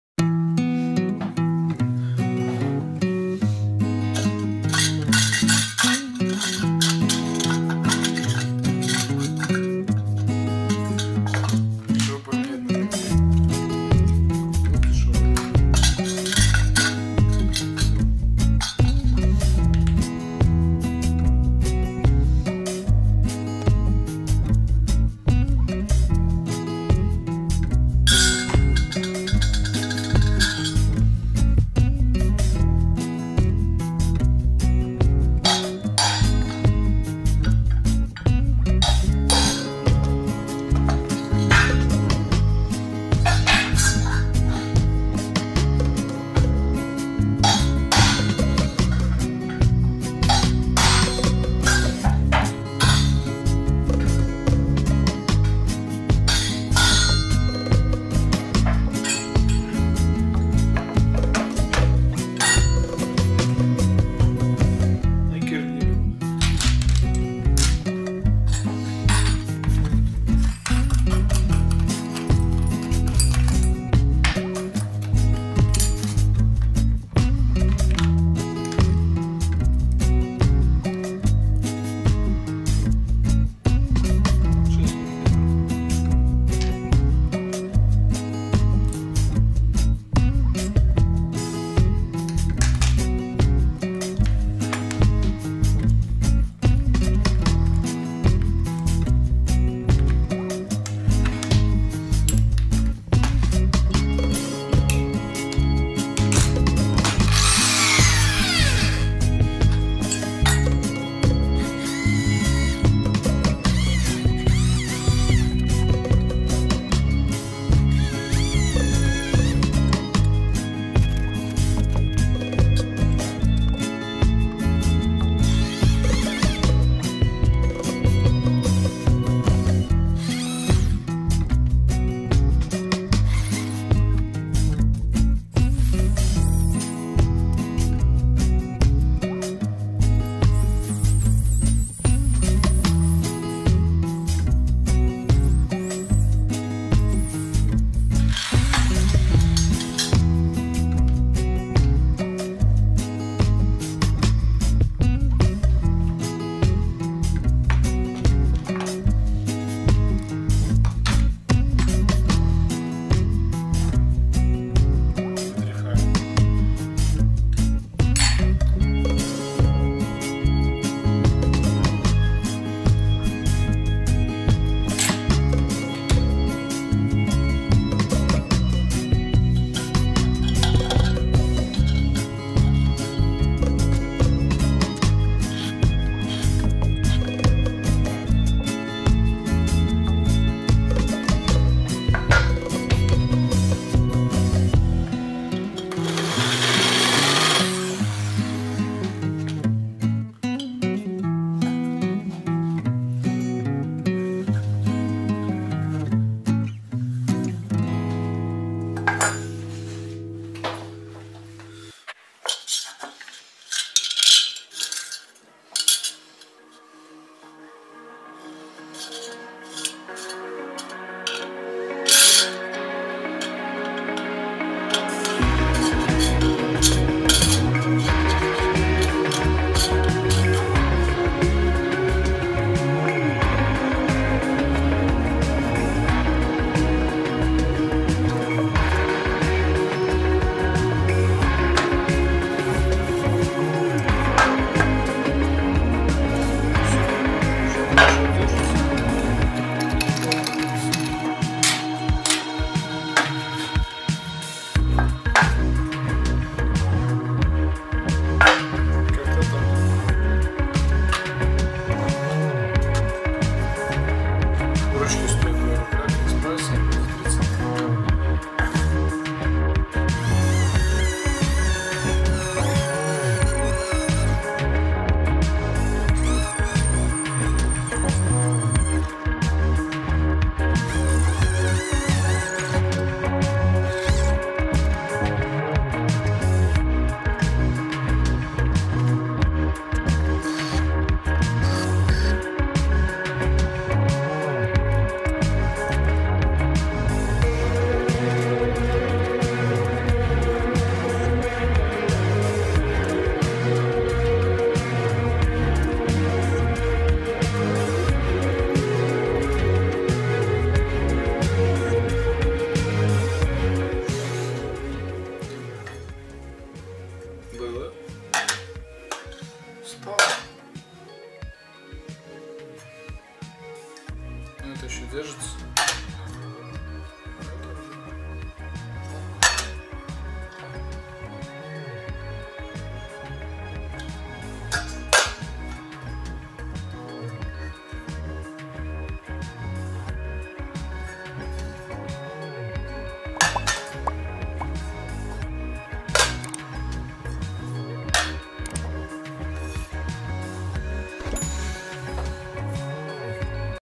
Замена ручек крышек.
Самостоятельная замена ручек на крышках кухонной посуды.